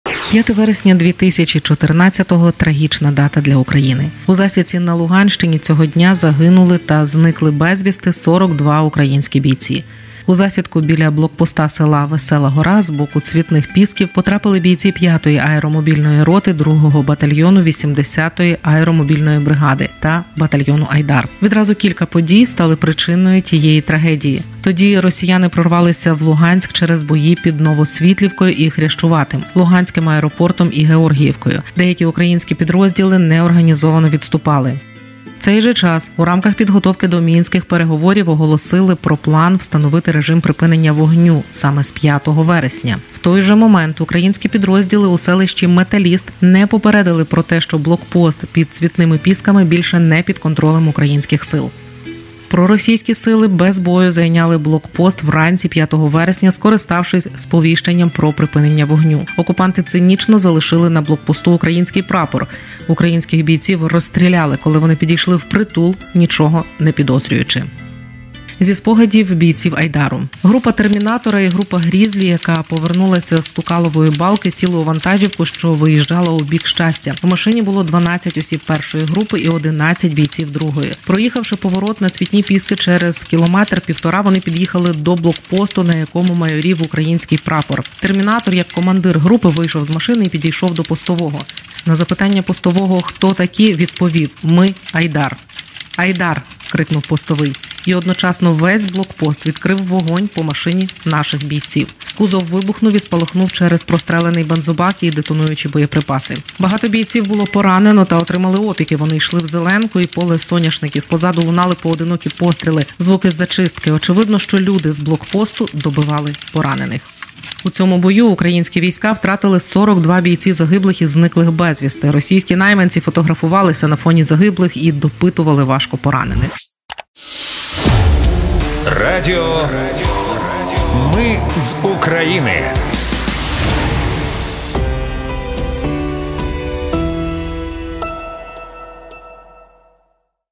5 вересня 2014 року - трагічна дата для України
5 вересня 2014-го  —  трагічна дата для України!
У засідці на Луганщині цього дня загинули та зникли безвісти 42 українські бійці

У засідку біля блокпосту села Весела Гора з боку Цвітних ПІсків  потрапили бійці 5-ї аеромобільної роти 2-го батальйону 80-ї аеромобільної бригади та батальйону «Айдар».  Відразу кілька подій стали стали причиною трагедії.

Радіо Ми з України - перемагаємо разом! 🇺🇦